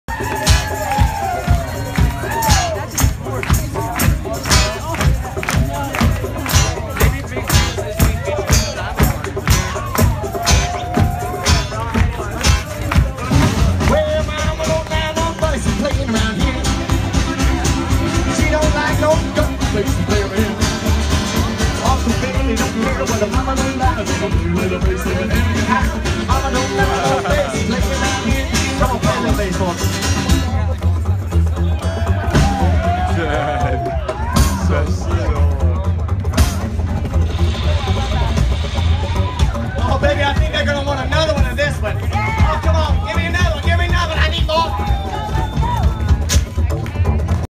The Great American Talent Show - Electric Forest 2015
These guys were amazing. I caught them a few times throughout my trip there. They kept getting cut off at the end of the night and there was a lot of weird things happening during their set. A girl got very moody and took the mic and ranted on about loving music. And then the sound guy kept cutting them off again. It was chaos but fun. Electric Forest 2015 on 6/28/2015.